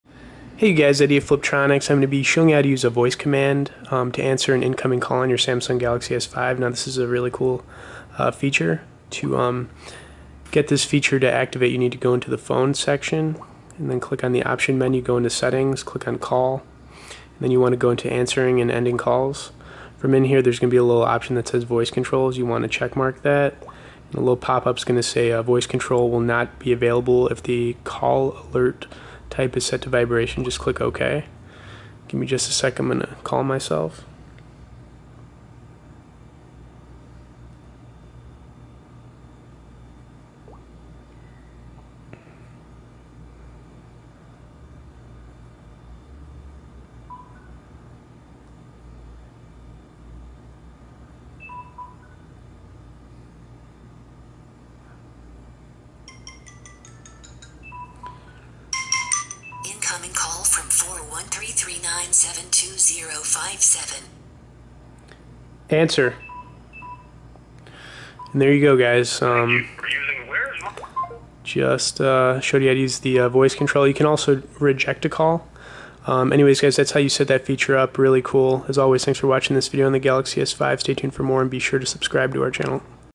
Samsung Galaxy S5: How to Answer / Reject a Call With Voice Command - Fliptroniks.com
Best Galaxy S5 Case
Best Galaxy S5 Battery
Best Galaxy S5 Charger

Using a voice command to answer an incoming call on your samsung galaxy s5 is a really cool feature that is easy to activate. To activate this feature go into the phone section and click the option menu and click settings. Once in the settings on your samsung galaxy s5 click on call, and then click answering and ending calls. Next  click voice control and make sure its checkmarked. To make sure it works have someone give you a call, and say the command answer, and it will automatically answer the phone call on your samsung galaxy s5.